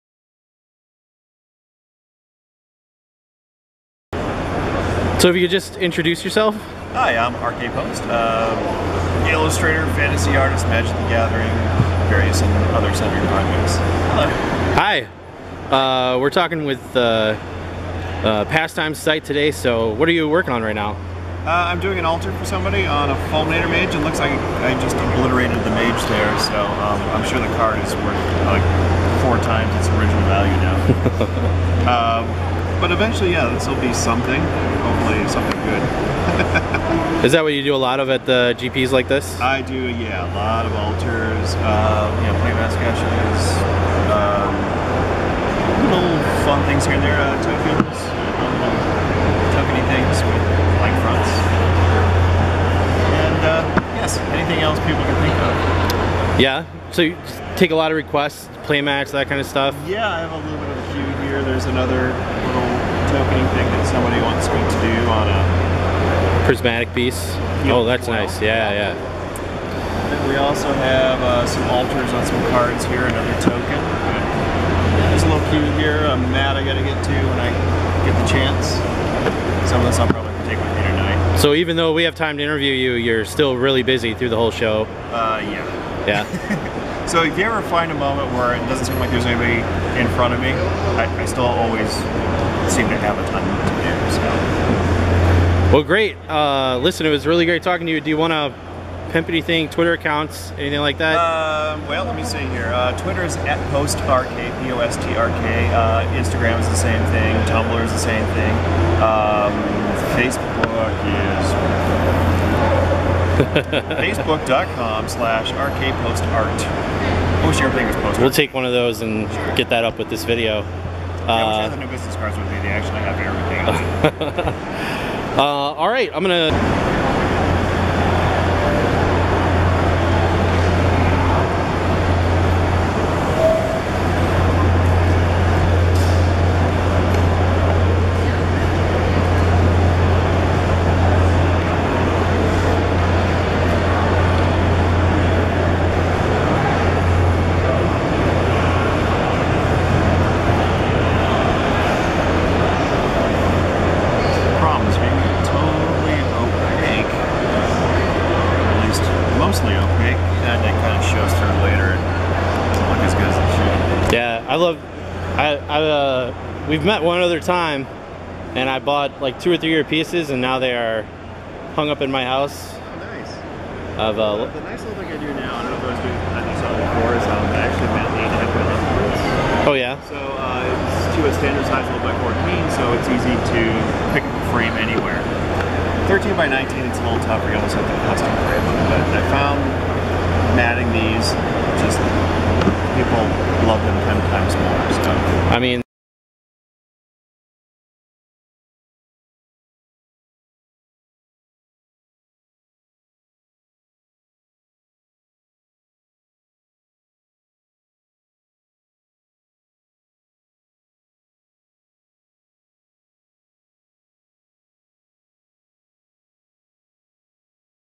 RK Post @
Thanks to RK Post for letting me interview him.  We chat a little about what he gets up to at events like GP Atlanta.  We see him work on some card alters and see his work available right her on site in Atlanta.